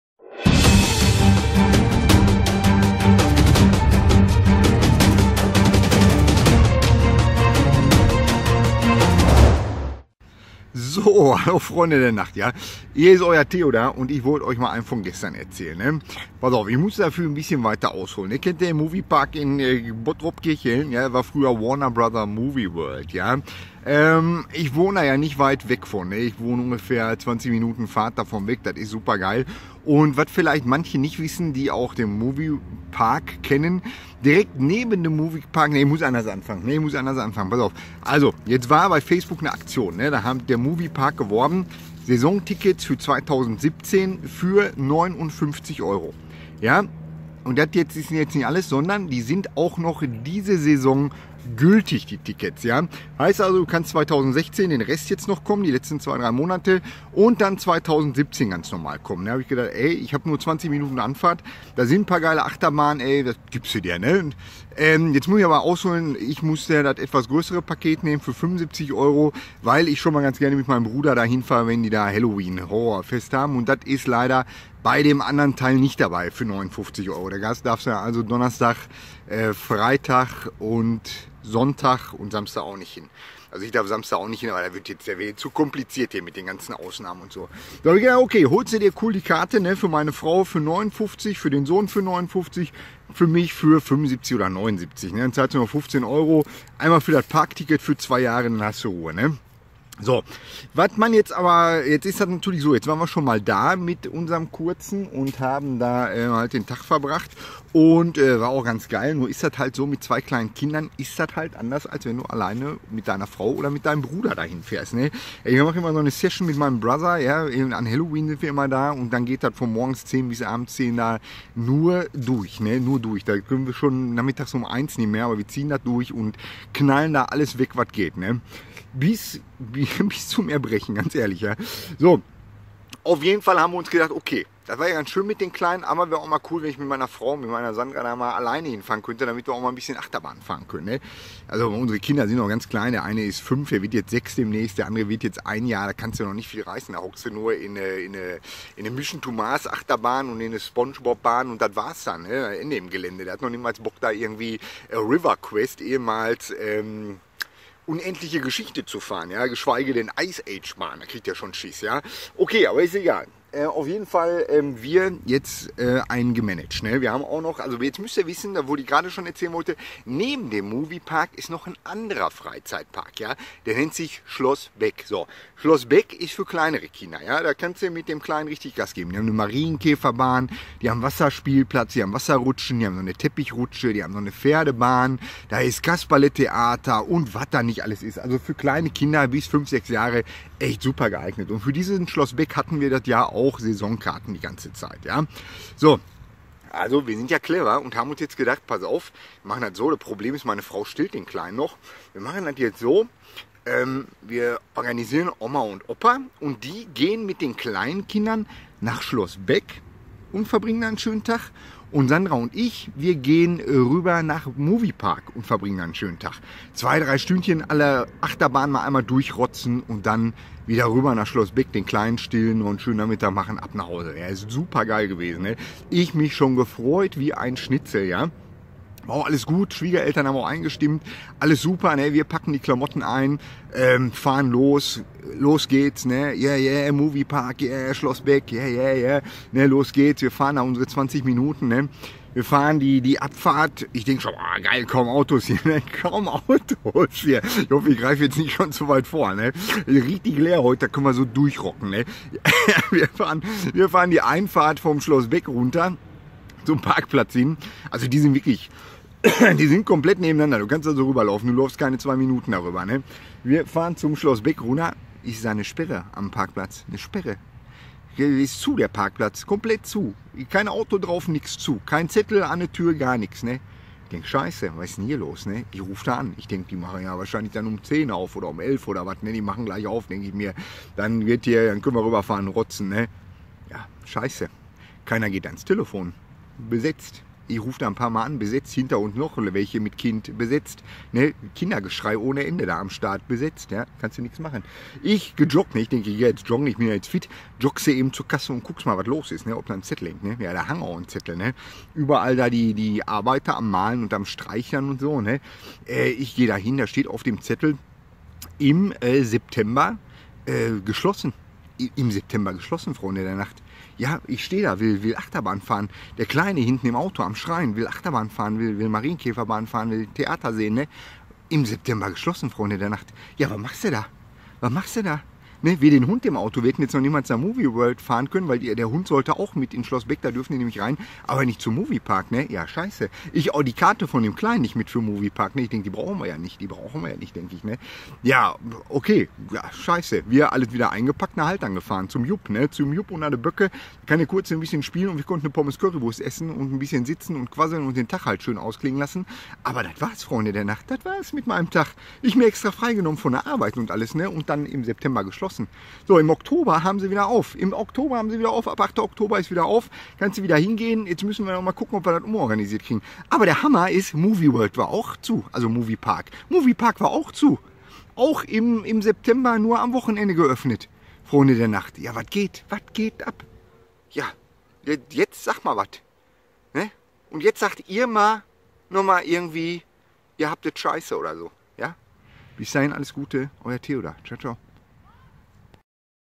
VLOG - Et lief alles nach Plan... Mein Tag im Movie Park!
Ab und an erlebe ich in meinem Leben einige Schoten die ich Euch nicht vorenthalten möchte.
So auch diese Woche als wir einen Besuch im Movie Park in Bottrop geplant hatten.
doch, seht einfach selbst was geschah!